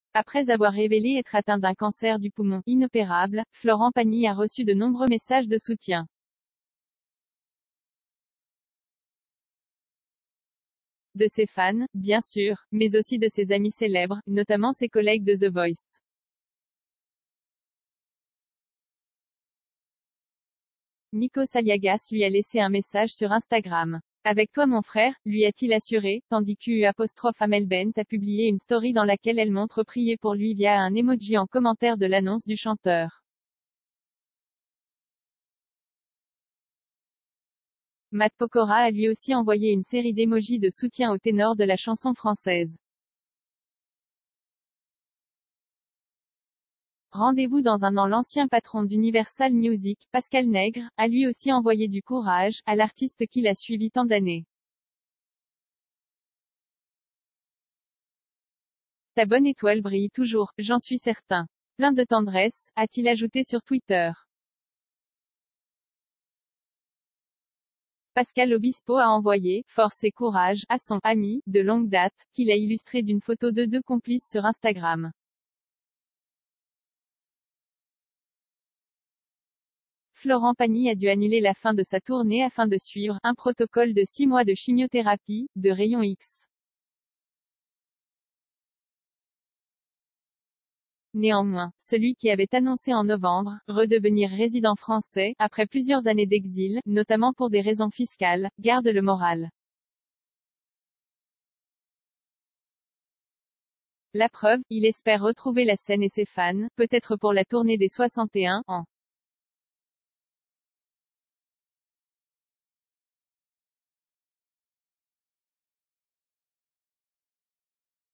Les stars soutiennent Florent Pagny depuis l’annonce de son cancer
Après avoir révélé être atteint d’un cancer du poumon « inopérable », Florent Pagny a reçu de nombreux messages de soutien. De ses fans, bien sûr, mais aussi de ses amis célèbres, notamment ses collègues de The Voice.   Nikos Aliagas lui a laissé un message sur   Instagram. « Avec toi mon frère », lui a-t-il assuré, tandis qu’  Amel Bent a publié une story dans laquelle elle montre prier pour lui via à un emoji en commentaire de l’annonce du chanteur.   Matt Pokora a lui aussi envoyé une série d’émojis de soutien au ténor de la chanson française.  Rendez-vous dans un an   L’ancien patron d’Universal Music, Pascal Nègre, a lui aussi envoyé du « courage » à l’artiste qu’il a suivi tant d’années. « Ta bonne étoile brille toujours, j’en suis certain !! Plein de tendresse », a-t-il ajouté sur   Twitter.   Pascal Obispo a envoyé « force et courage » à son « ami » de longue date, qu’il a illustré d’une   photo d’eux deux complices sur Instagram.   Florent Pagny a dû annuler la fin de sa tournée afin de suivre « un protocole de six mois de chimiothérapie, de rayons X ». Néanmoins, celui qui avait annoncé en novembre « redevenir résident français » après plusieurs années d’exil (notamment pour des   raisons fiscales), garde le moral. La preuve : il espère retrouver la scène et ses fans « peut-être pour la tournée des 61 » ans.